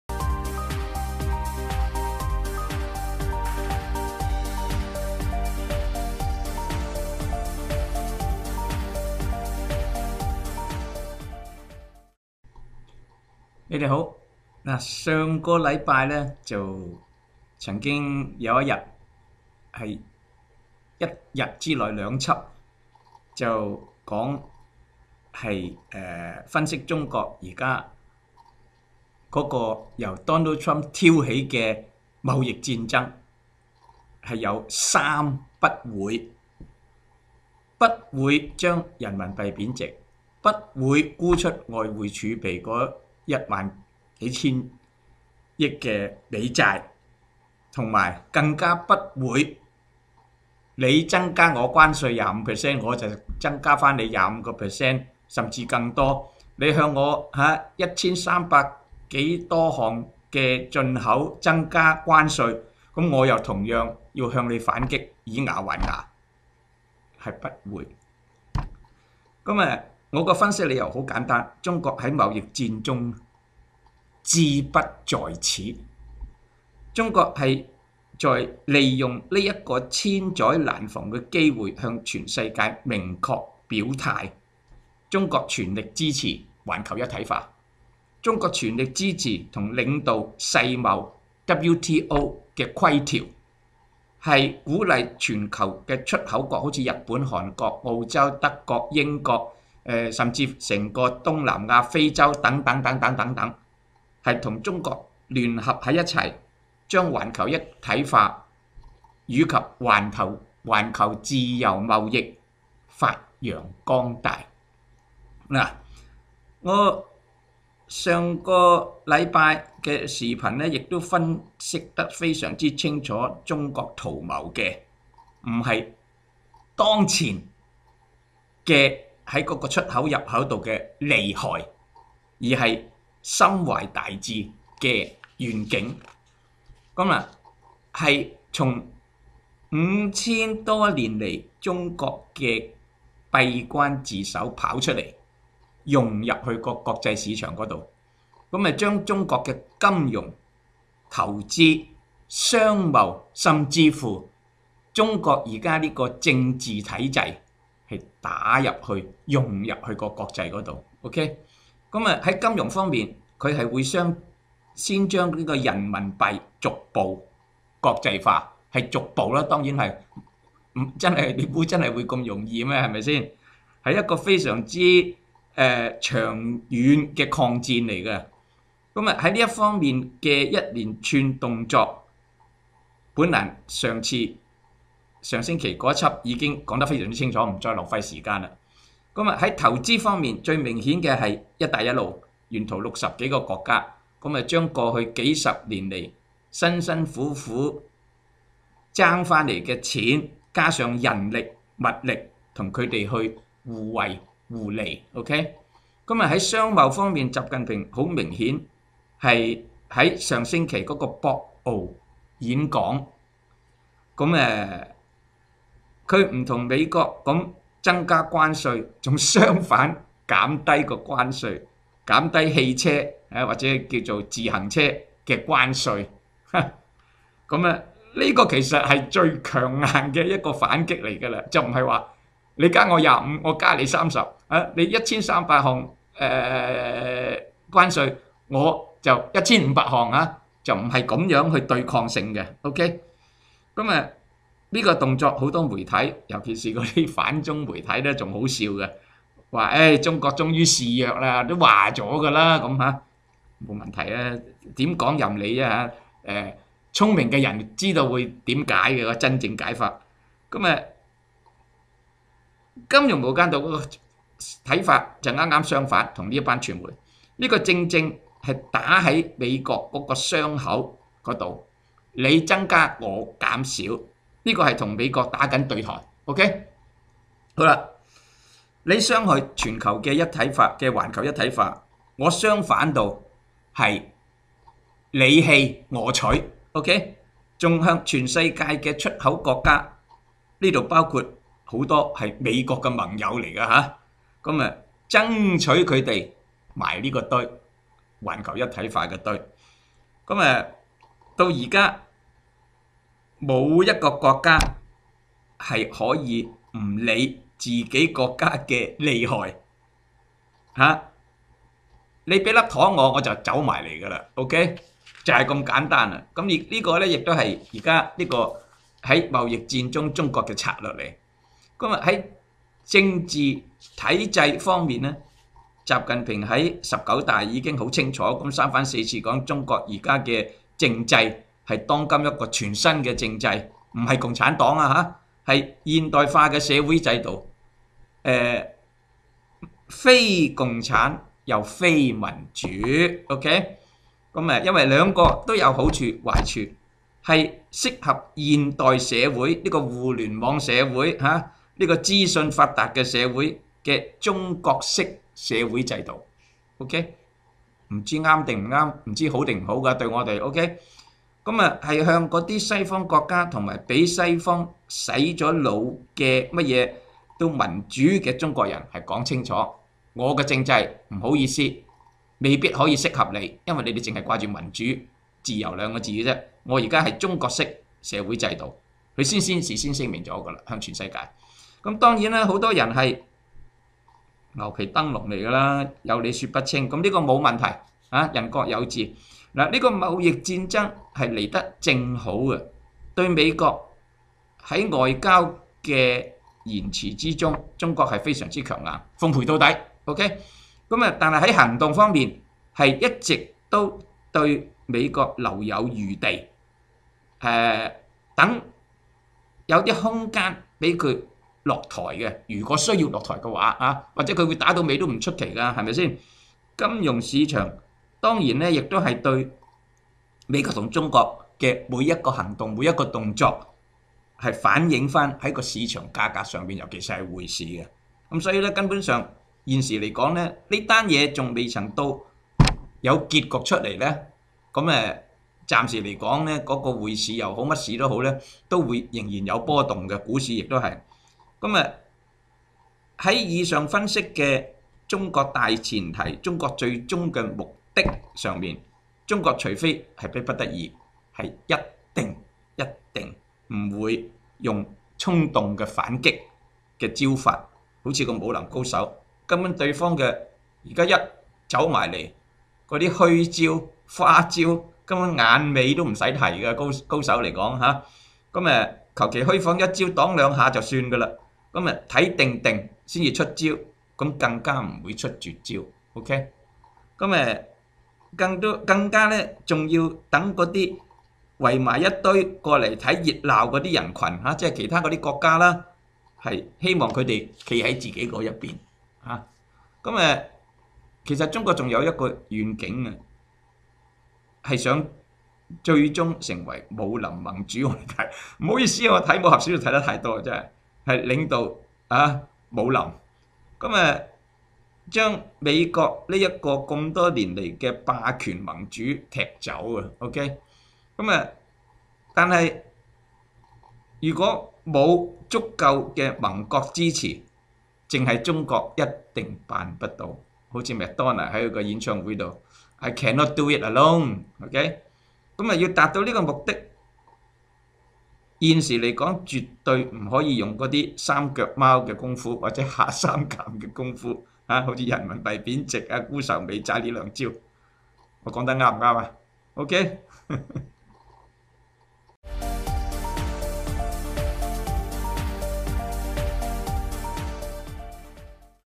20180416 金融無間道：中美貿易戰志不在關稅 那又是為了甚麼？
外匯專家 何保 再談中美貿易糾紛，關稅未必是雙方最大的執著點，更只是手段而非目的！那中美兩國又在打甚麼算盤？請去片足本評論～